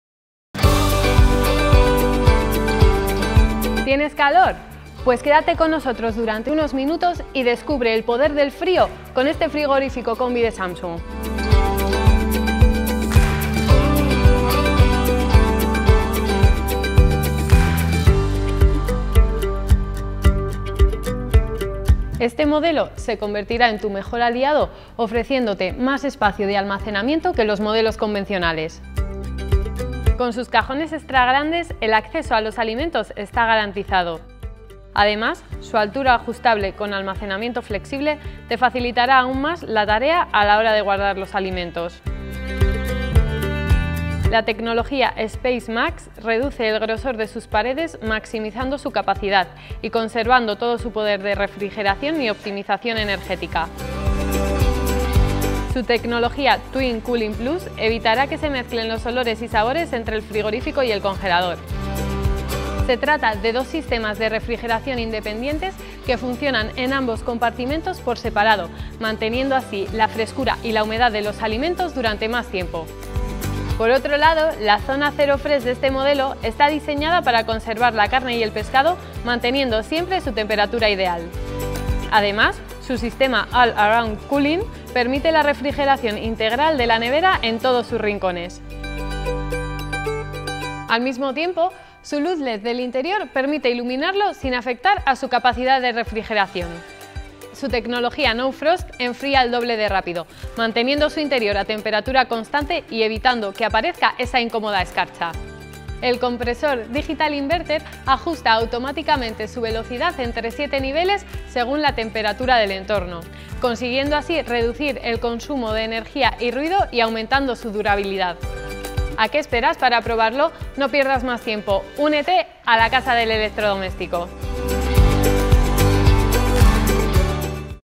❄ Samsung frigorífico combi | RB37J501MSA
Presentamos el ❄ frigorífico Samsung ❄ RB37J501MSA, No Frost en color inox. Con una eficiencia energética A+++ y con una capacidad de 353 litros (19 bolsas). Medidas: 201x59.5x67.5 cm.
Contenido:
presentación - 0:00
características técnicas - 0:12
interior - 0:22
tecnología SpaceMax - 0:44
tecnología Twin Cooling Plus - 0:55
zona 0º fresh - 1:13
all around cooling - 1:21
despedida - 1:59